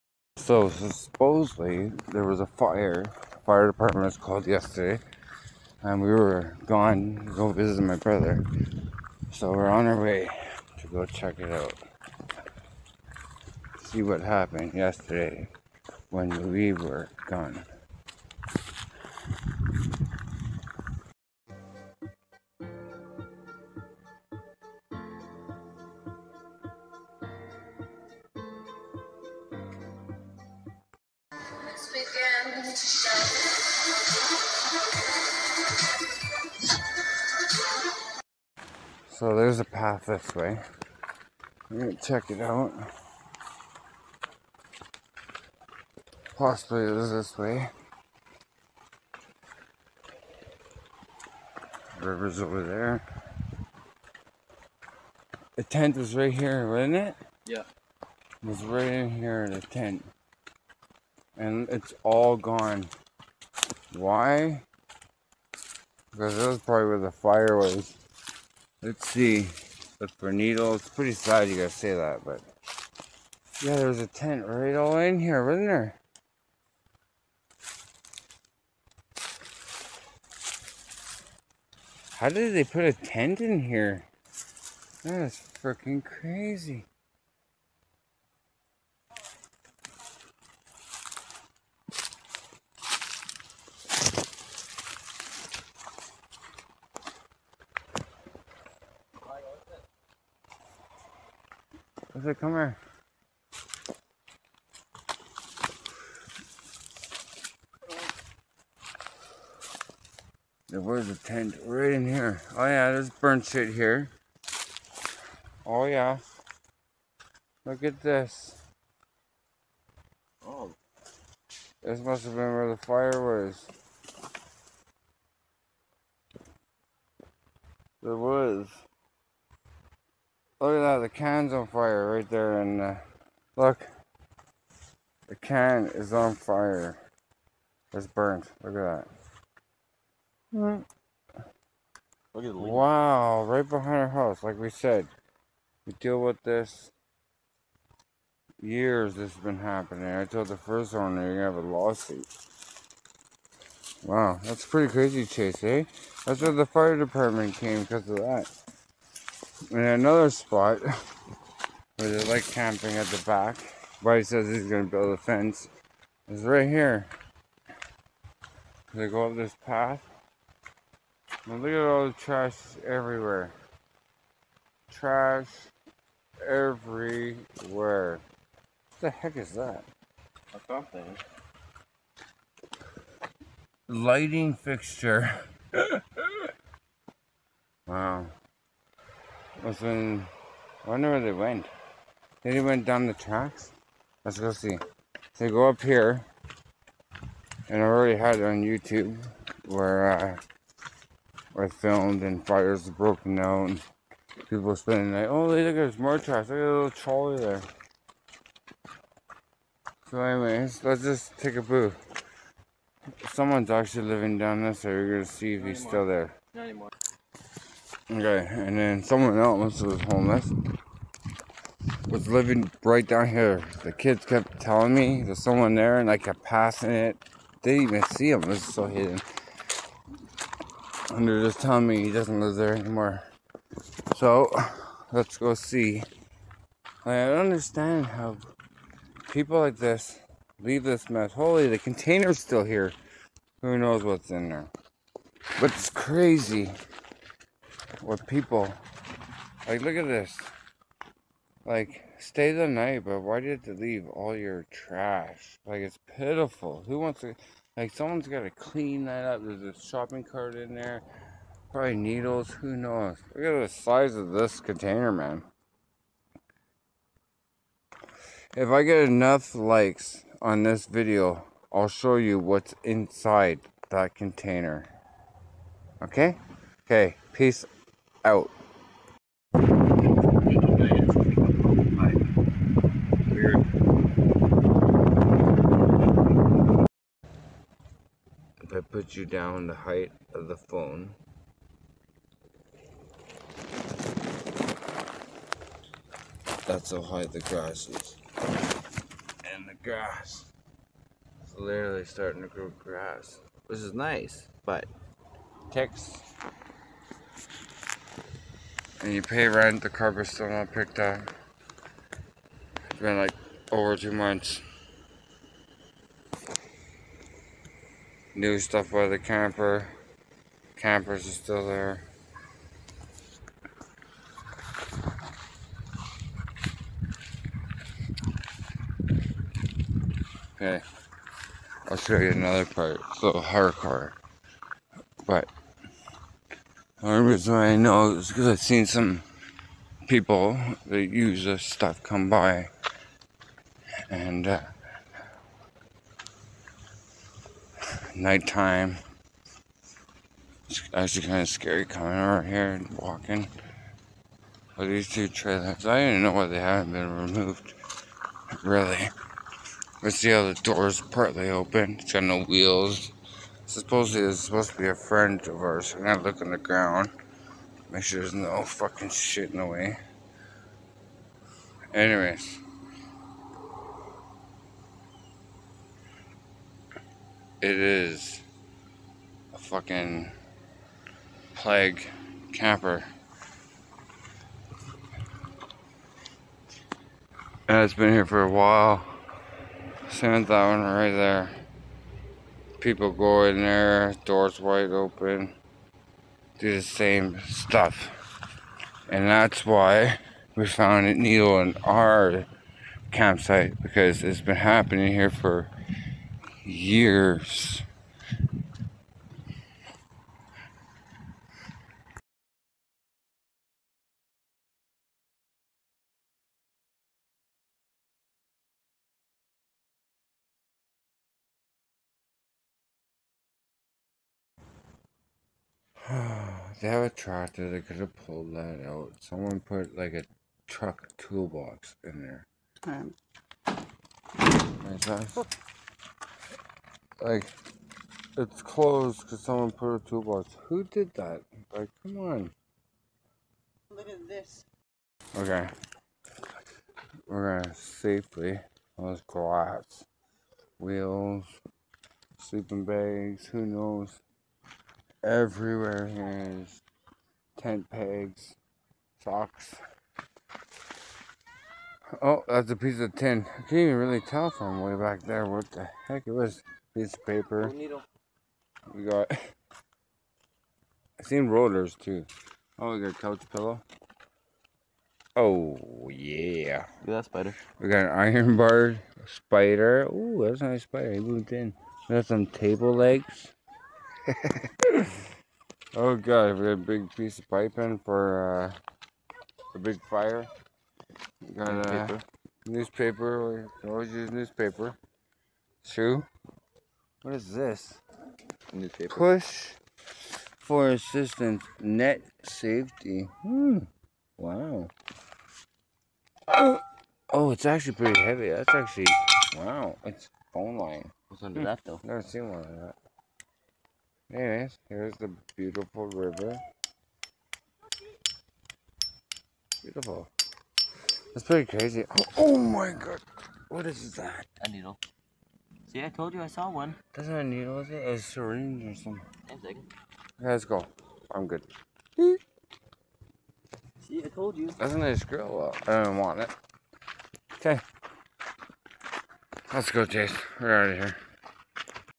Episode #7 a fire breaks out behind our RV when we were gone👀👀
First time going to Nanaimo in about a year and a fire breaks out when we were gone behind our RVs, also a little bit more information on the status of the campground and other things hidden that you don't see on the news.